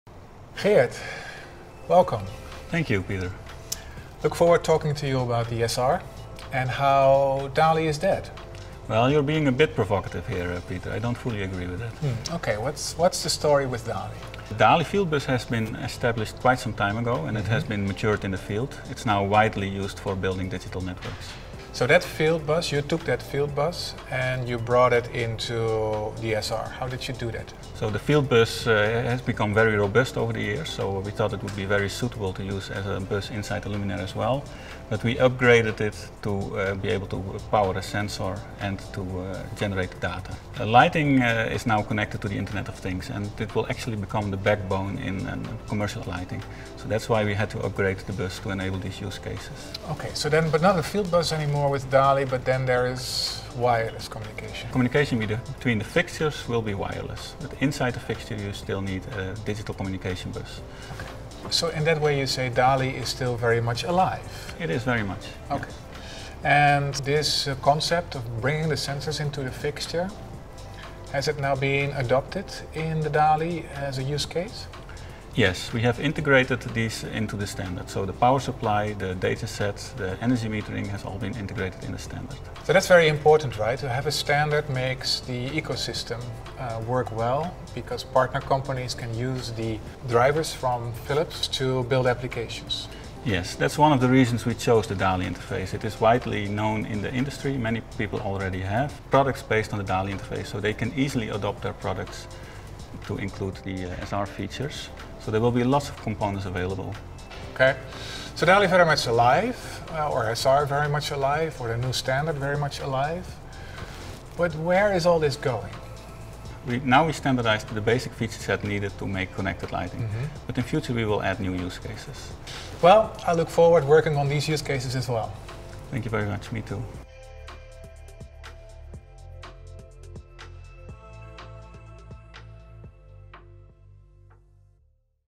What's next, DALI?
Watch Peter Duine and Geert van der Veen discuss how the Digital Addressable Lighting Interface (DALI) evolved into the new sensor-ready (SR) interface to provide a wireless-enabled infrastructure for the Internet of Things (IoT).